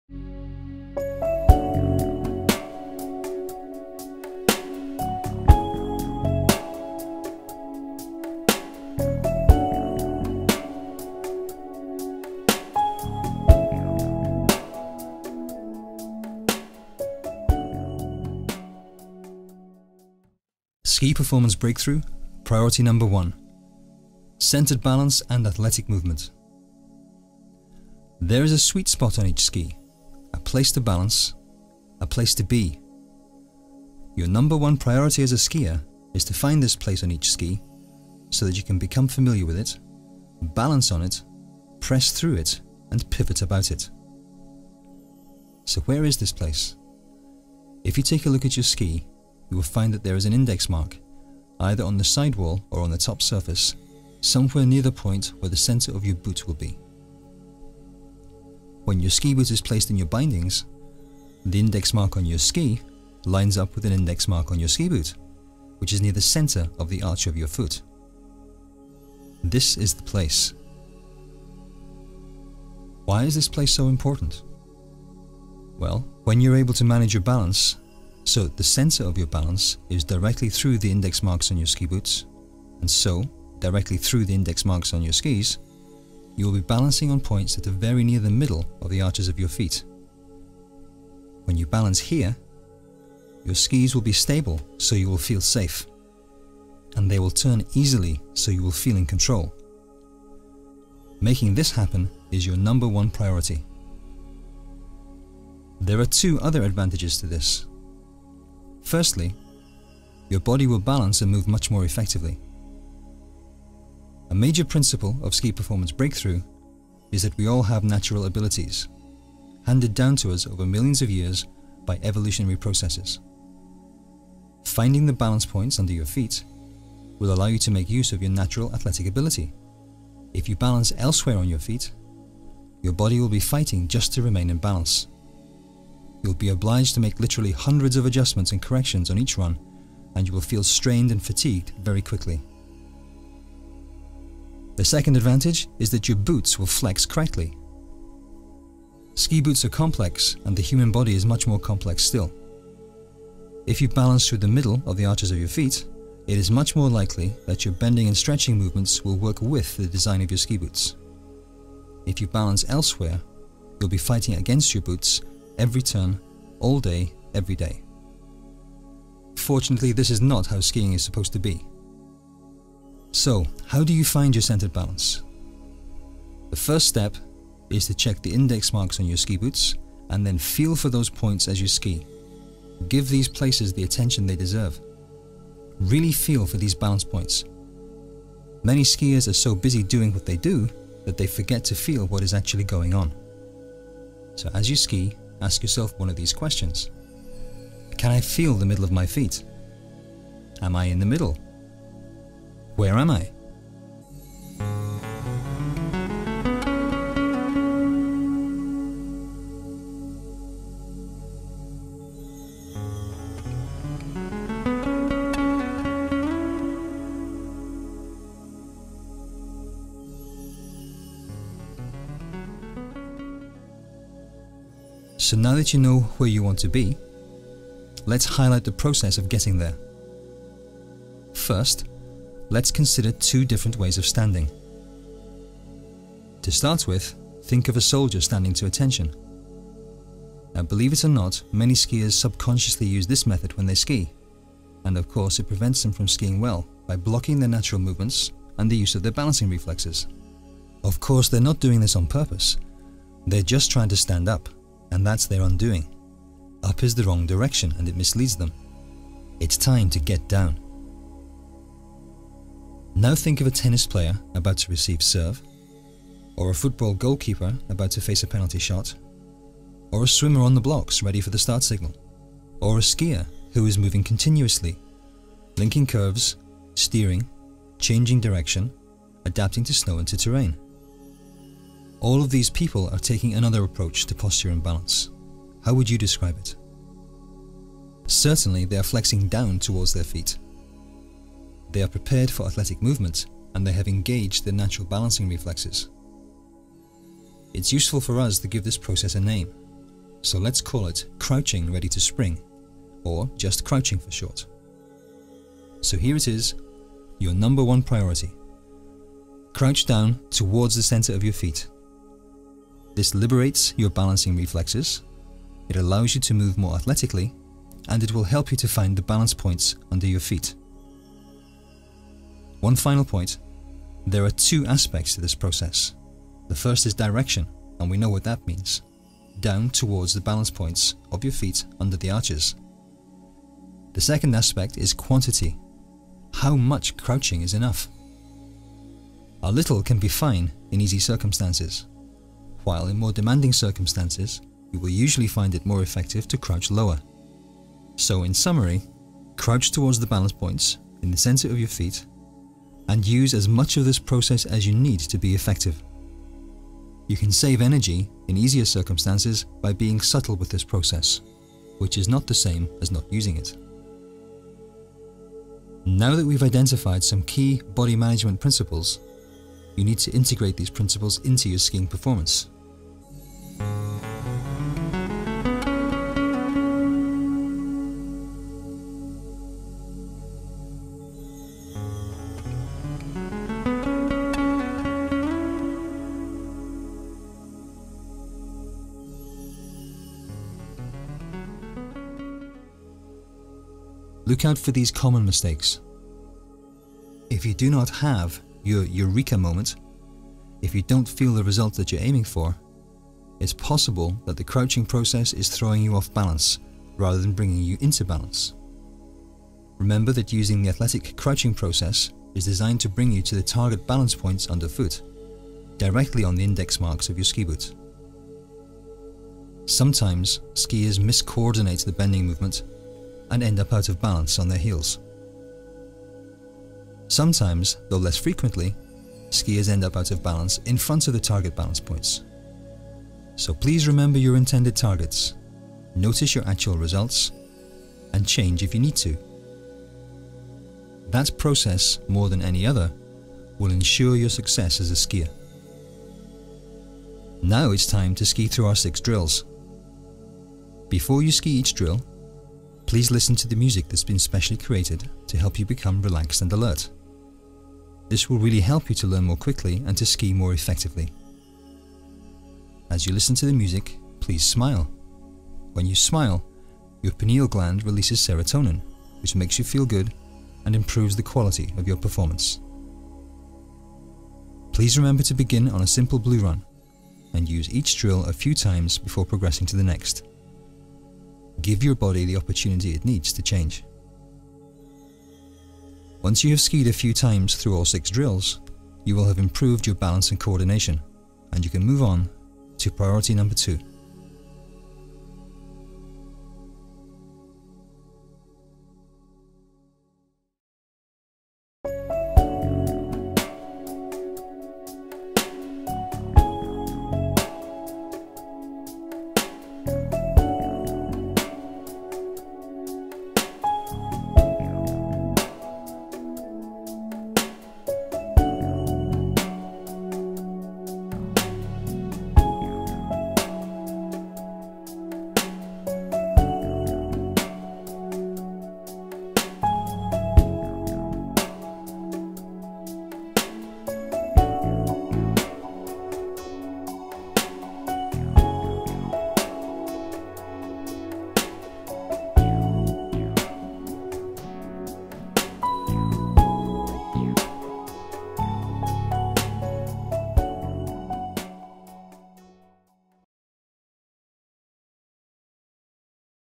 Ski Priority 1 - Centred Balance
Balance - your number 1 priority for great skiing, shown, explained and put to use. Playlist of 8 related videos in this series